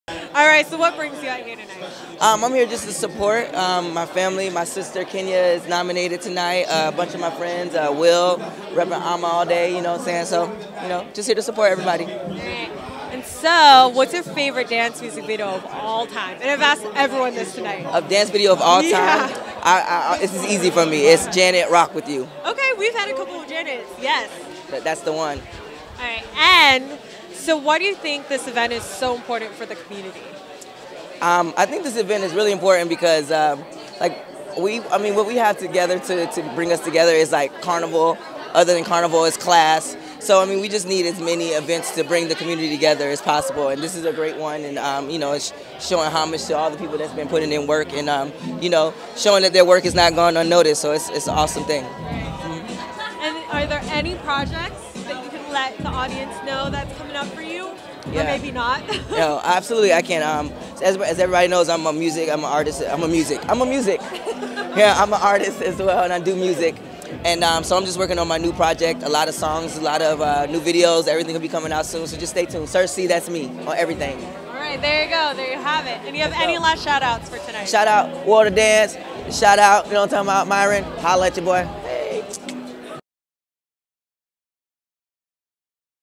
Sir Charles show his support for dance community at WOD 2016
Pacific Rim Video correspondent Jenny Valles talks to Sir Charles during the red carpet arrivals of the 2016 World of Dance Awards held at The Avalon in Hollywood, CA on Tuesday, April 9, 2016.

Photos by Sthanlee B. Mirador.